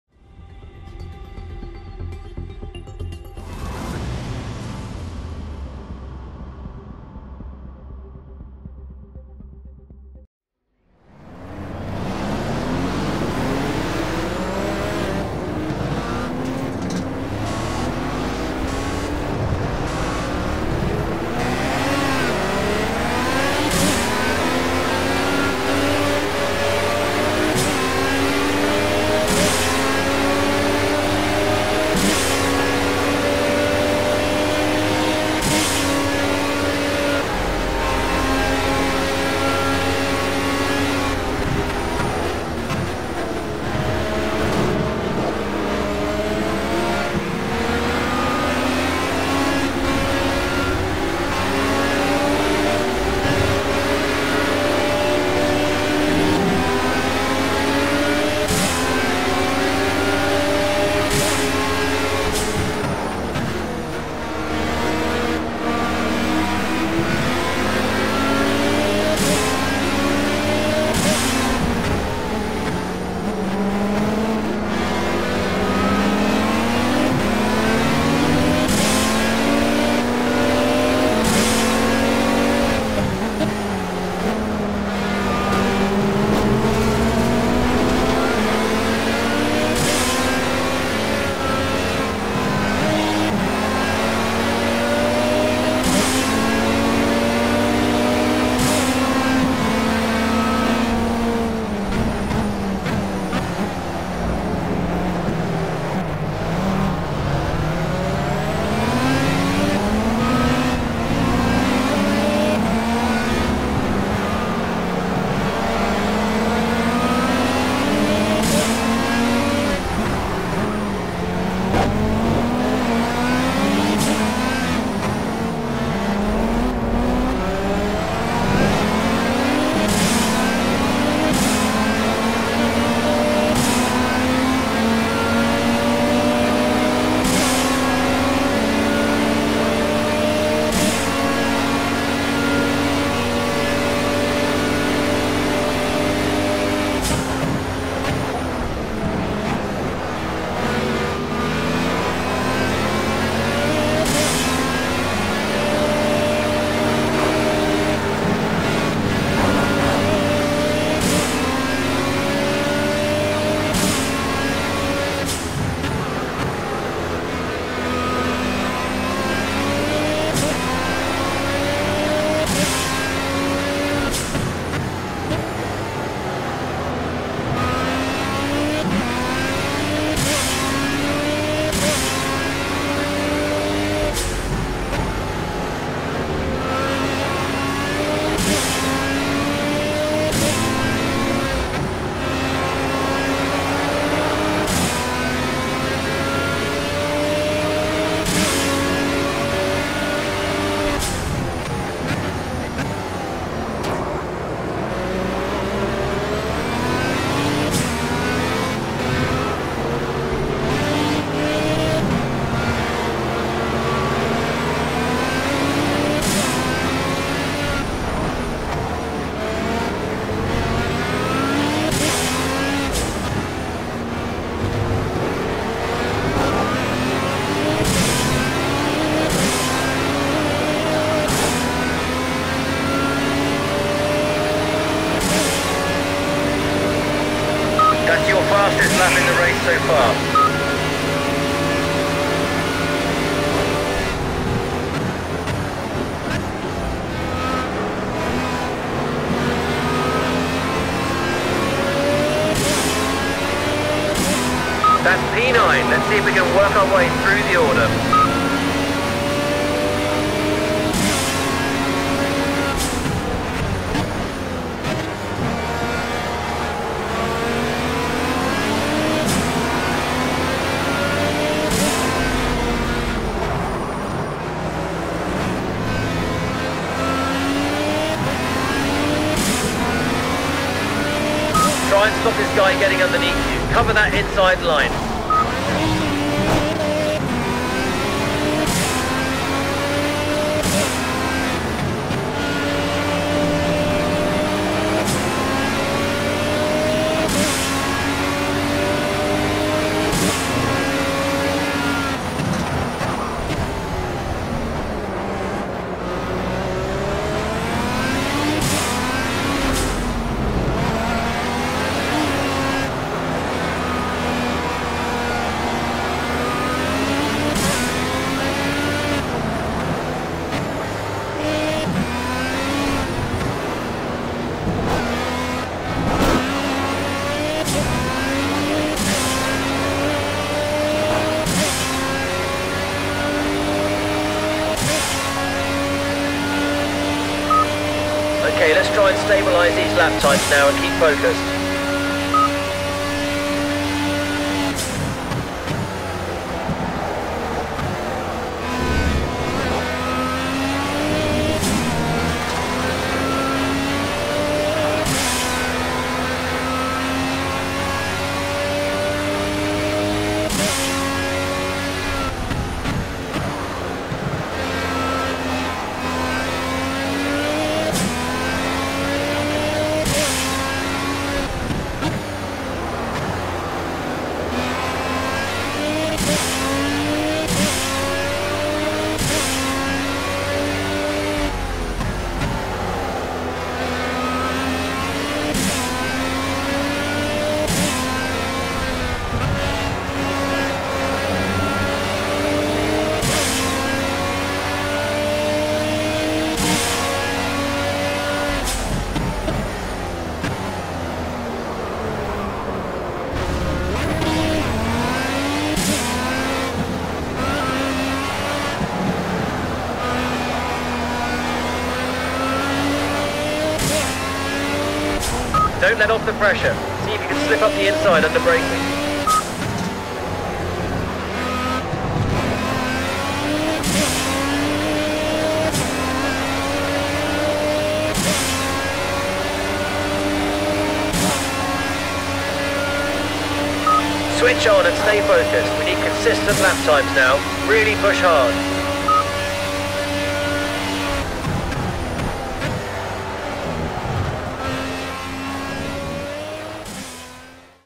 First 4 Laps of Barcelona GP in a RWD P30 LMP1 90%
First 4 laps at the Barcelona GP. Only showing the first 4 beacuse I got into 4th place and stayed there for the rest of the race. Race leaders were 12+ Seconds.

Vehicle was the RWD P30 LMP1
Race was at Barcelona GP track, 20 laps, 30 multiclass racers, 90% AI difficulty. Race started at 17:00 at a 5x time progression and weather that went from light cloud to clear synced with the race.

Running HUD Dash on my phone to keep an eye on my entire car at once so I don't have to switch between my wheel LCD every 3 seconds. HUD Dash can be found here, It has a free version but its not that great, Highly recommend buying it for $7.